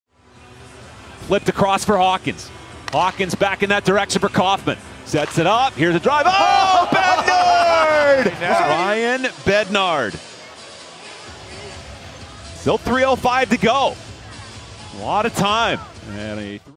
Save of the Week- Newark
Ryan Bednard was flashing the leather in Newark! Here's your Hoser Hockey Save of the Week.